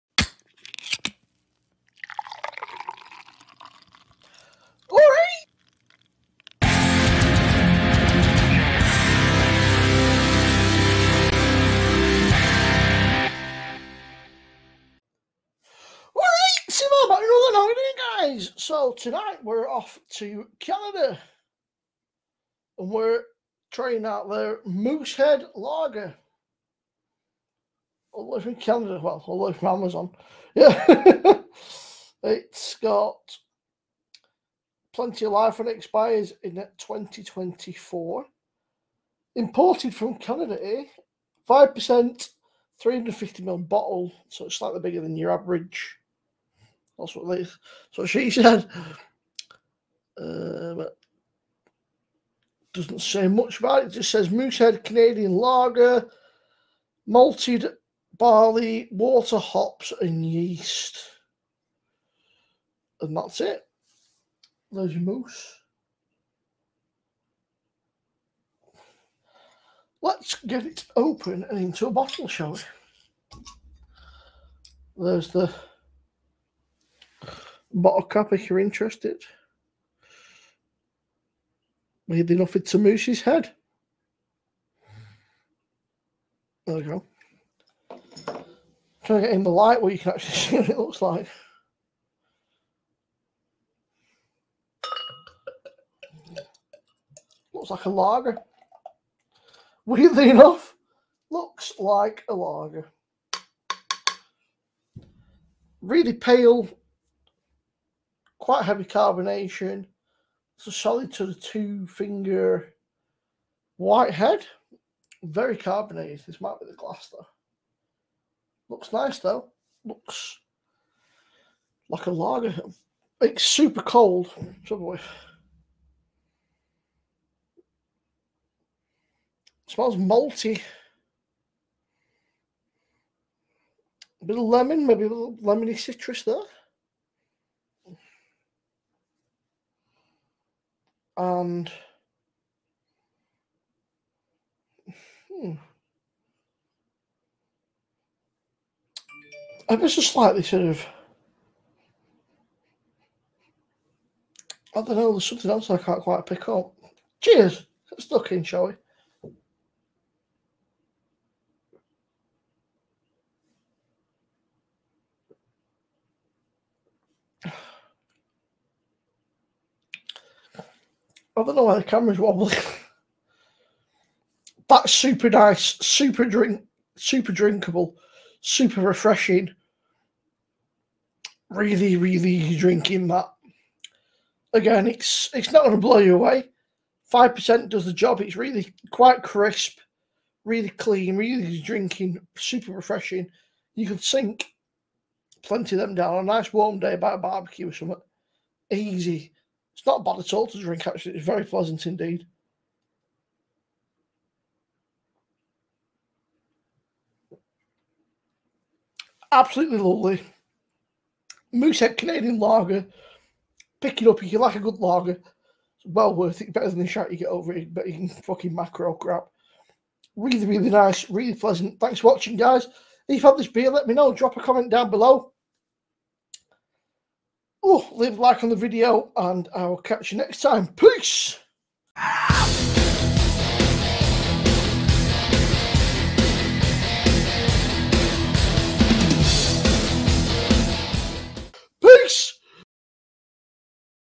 Moosehead | Canadian Lager | 5%
Sold throughout Canada, the United States, and in select countries around the world, our flagship Moosehead Lager is often regarded as Canada’s premium lager. This golden, refreshing lager offers a fine balance between malt sweetness and hop bitterness. Brewed with 100% Canadian two-row pale malt and Moosehead’s own lager yeast, Moosehead Lager is cool-fermented and cold-aged to impart its award-winning flavour.